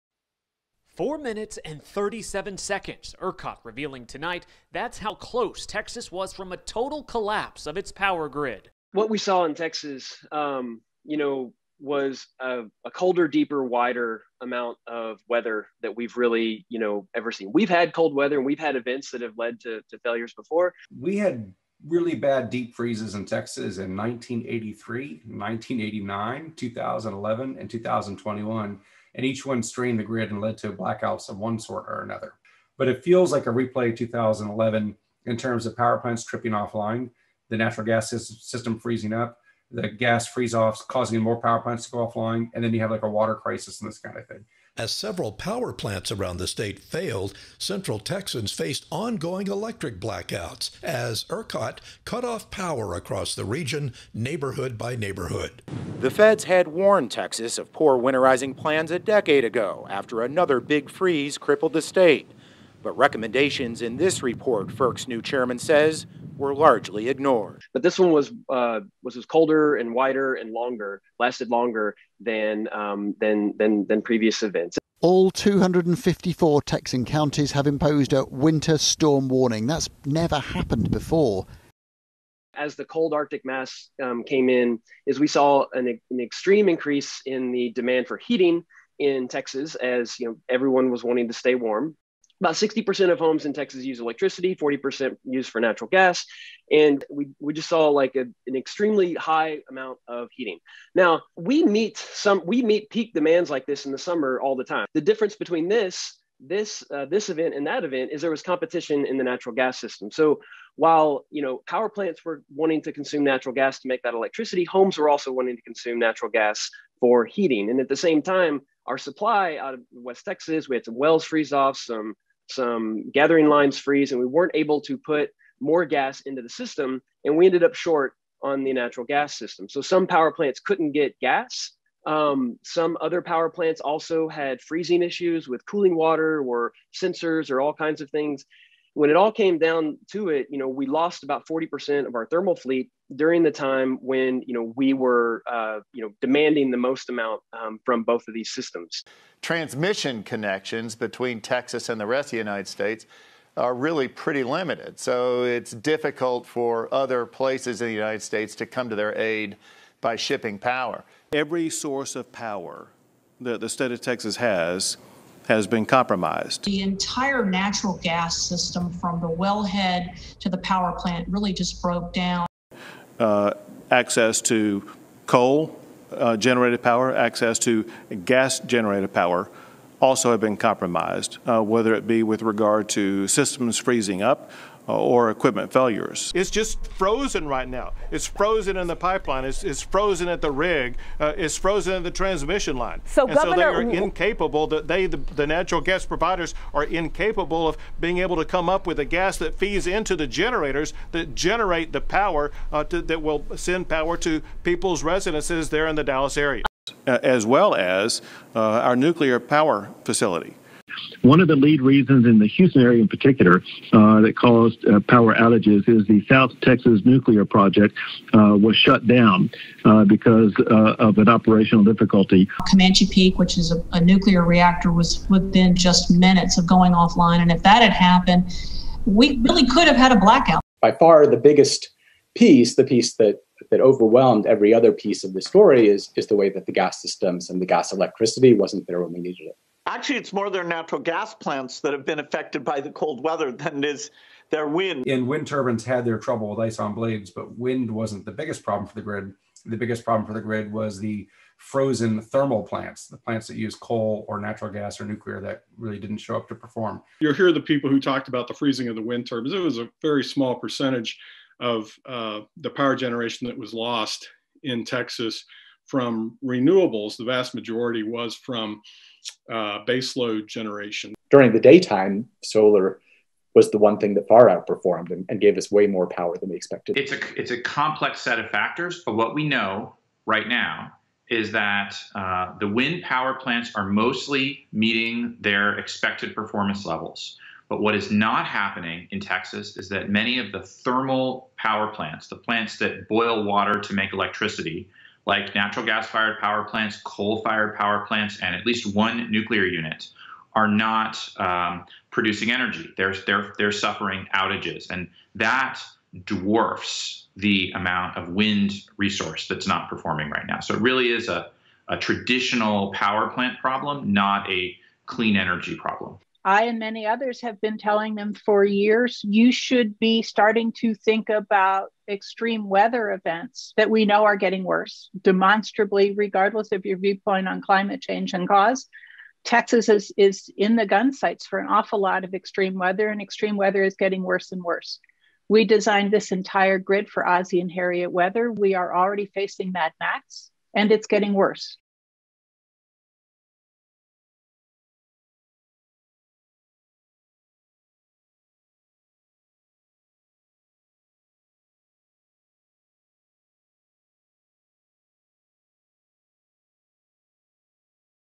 Experts on the Texas Blackout
Concise expert analysis from University of Texas and Rice University researchers, as well as the best journalistic reporting on the Texas Blackout disaster.